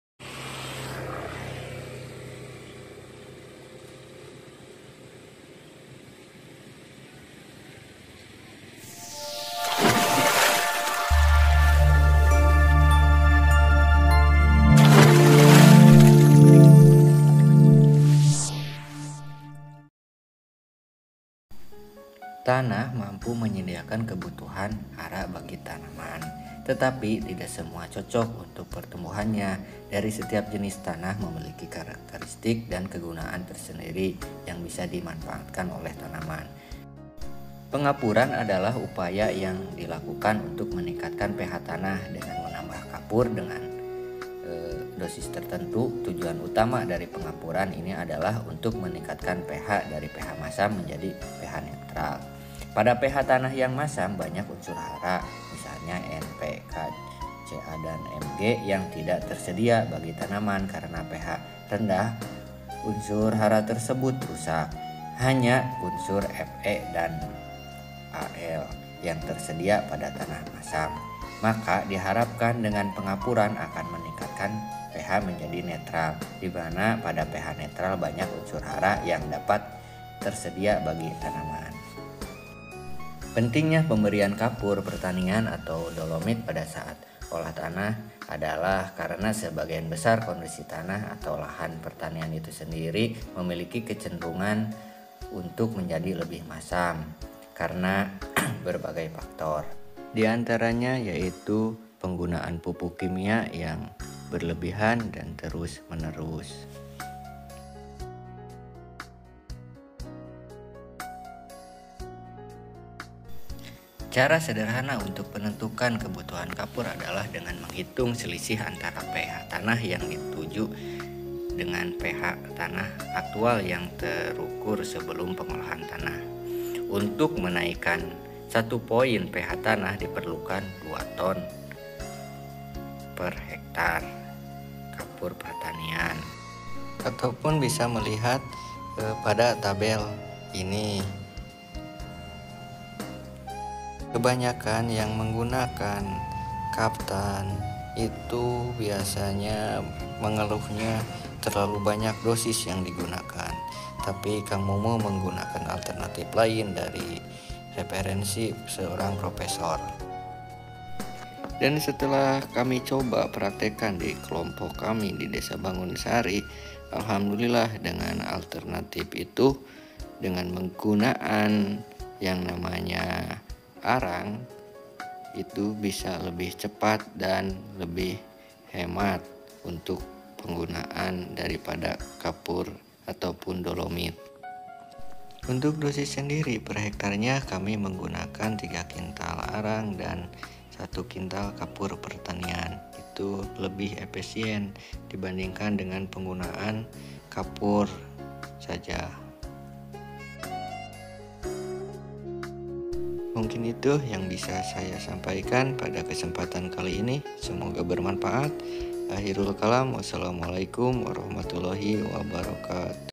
Cuman Dengan Ini, PH Tanah Langsung Naik
Tanah yang masam akan menurunkan produksi dan gampang terserang hama dan penyakit maka dengan pemberian kaptan akan meningkatkan PH tanah tapi dengan penggunaan kaptan banyak mengeluh dengan dosis yg terlalu banyak, kang mumu memberi alternatif untuk itu dengan 300 kg plus 100 kg kaptan /ha cukup untuk menaikkan PH